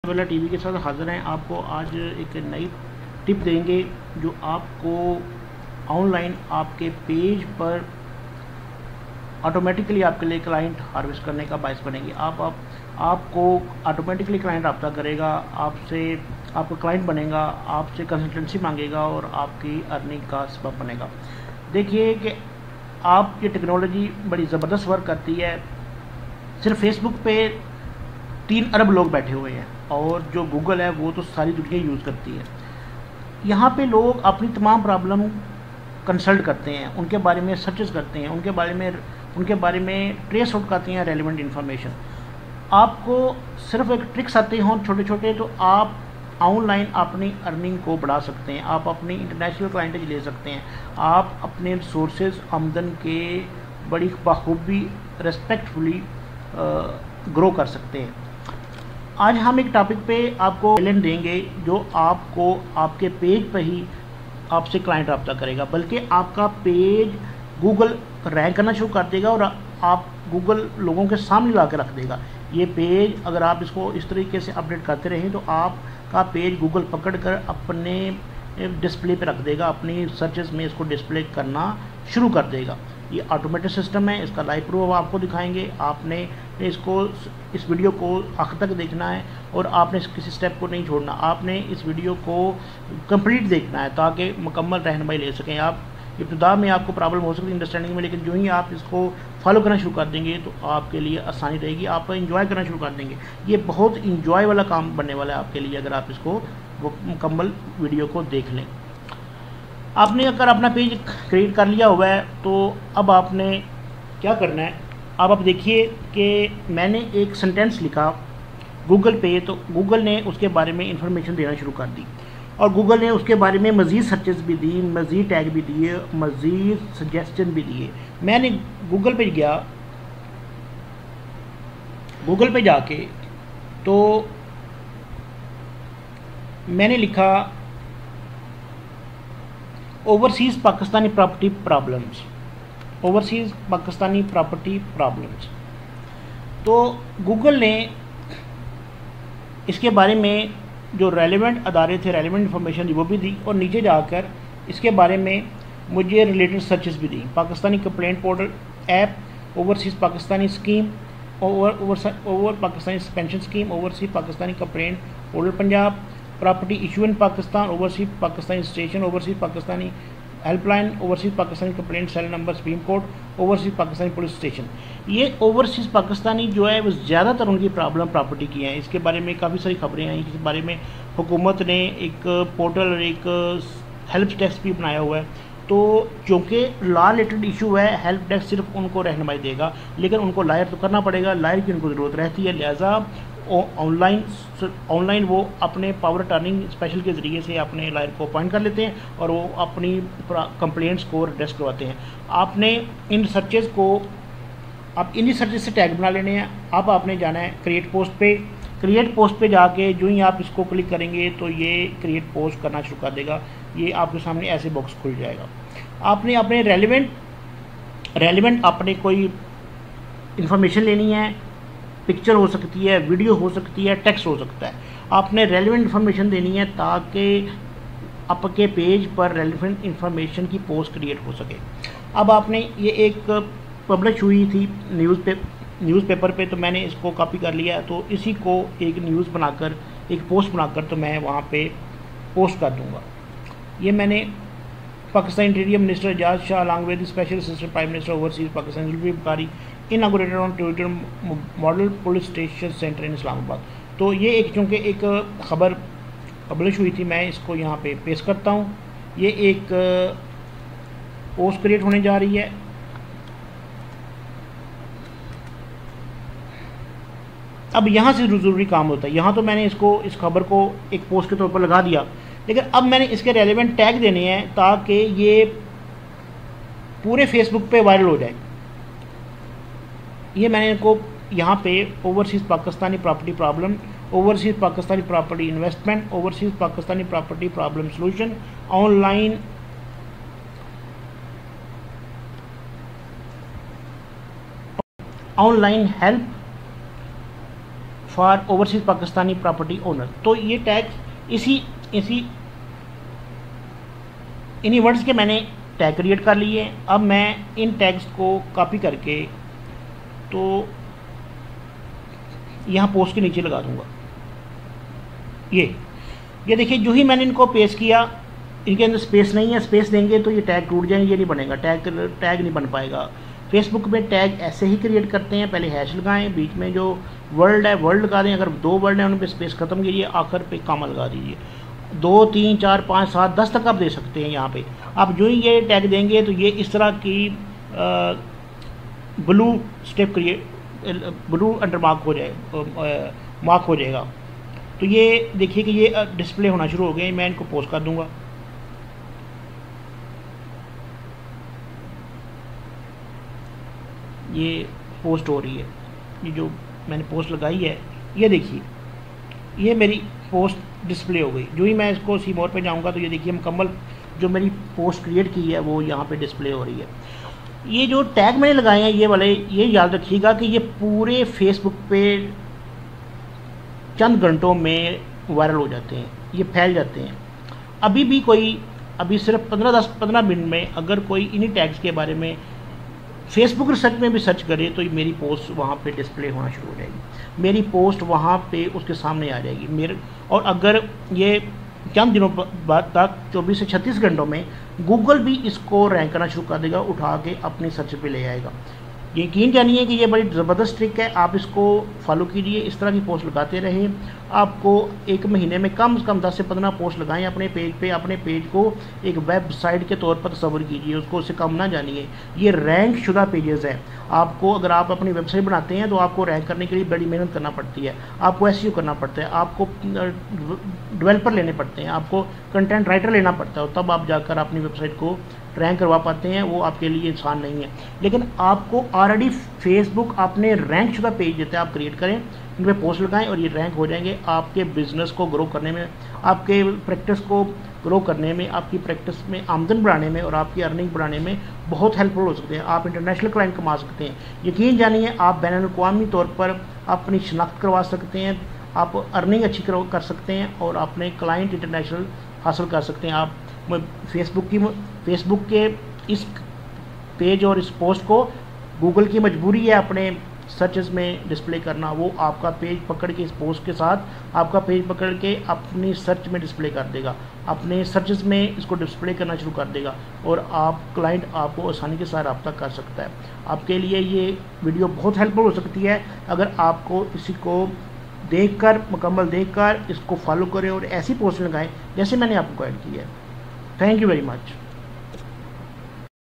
Overseas Pakistani Clients May Contact || Lawyer Should Create Posts This Way|| Cyber Law TV
There are huge opportunities to earn clients while using Facebook. Facebook pages work like websites. By creating posts with hashtags, lawyers may harvest clients.